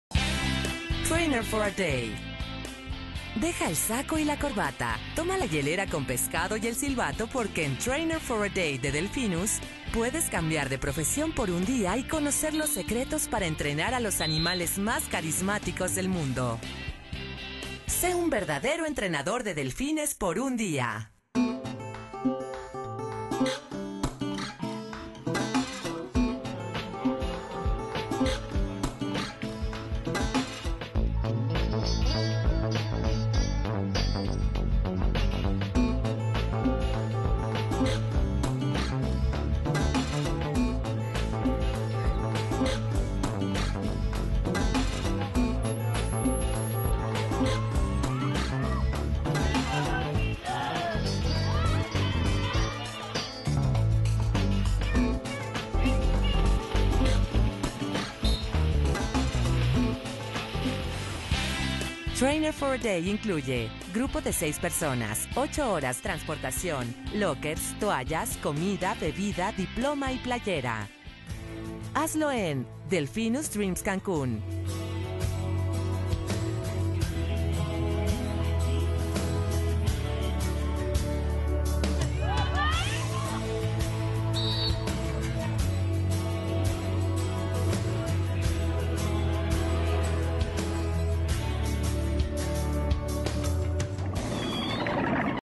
Nado con delfines - Entrenador por un dia
En este programa podrás comunicarte con los Delfines de la misma manera como lo hace un entrenador profesional. Conocerás con mayor profundidad detalles sobre estos brillantes mamíferos: su anatomía, alimentación, comportamiento, técnicas de entrenamiento y cuidados necesarios para mantener su salud en óptimas condiciones.